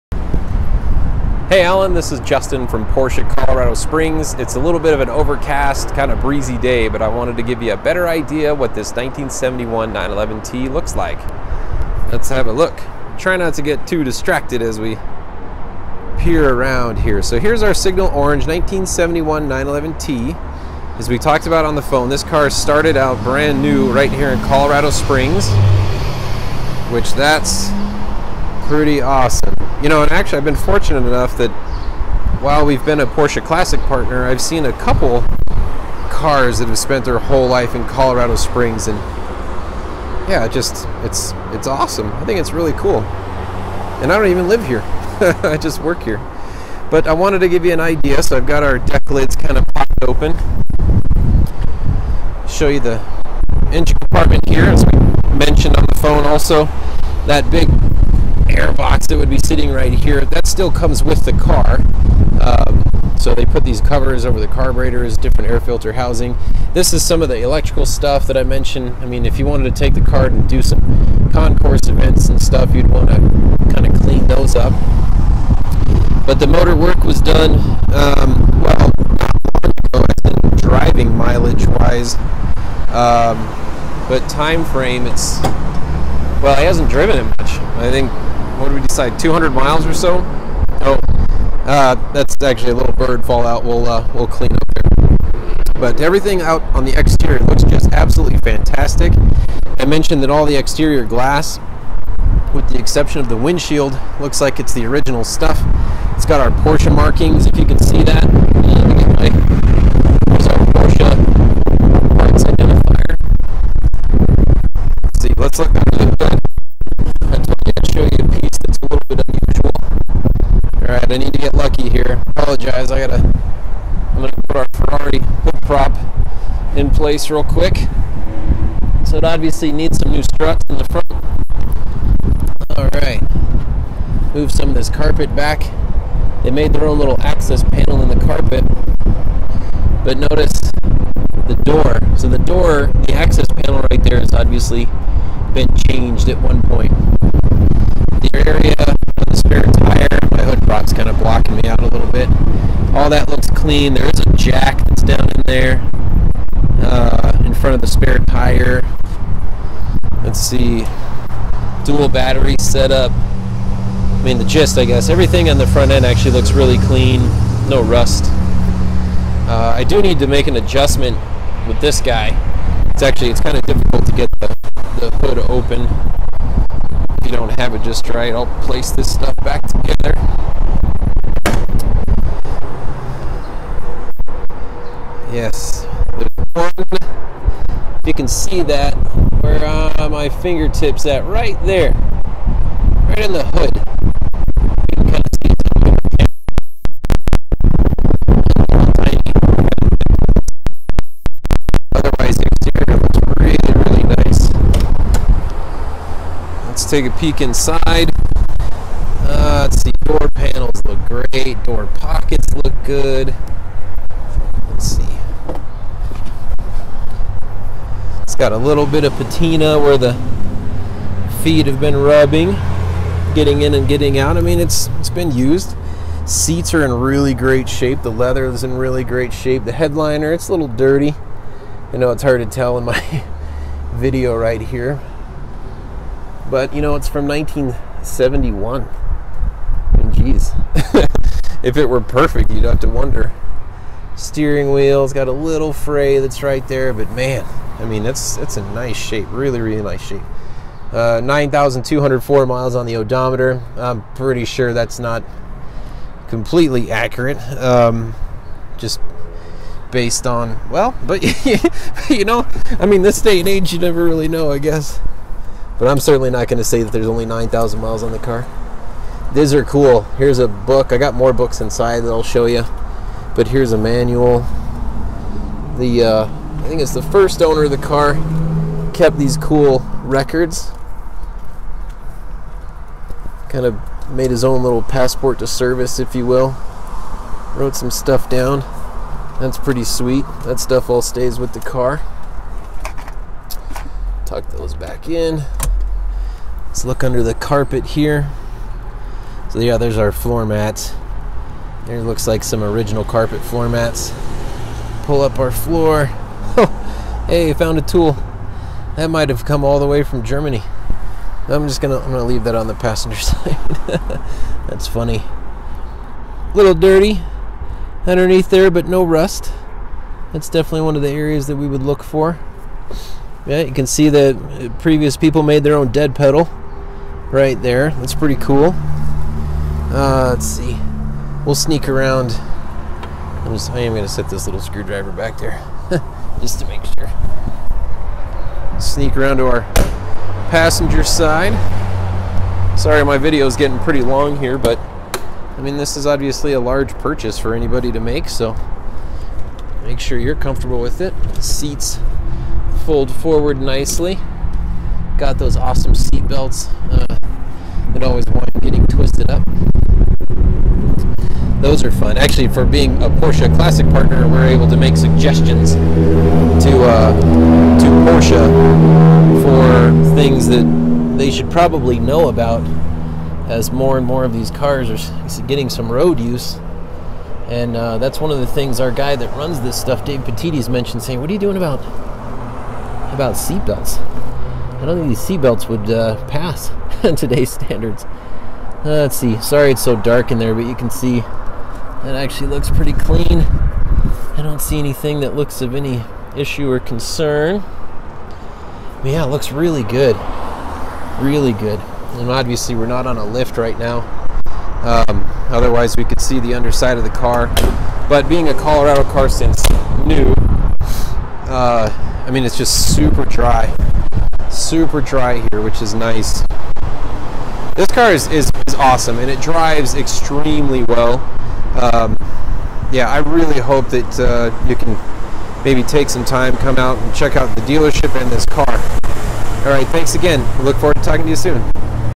1971 911T with Justin
Walk around of a Signal Orange 911T for sale at Porsche Colorado Springs with Justin Underwood.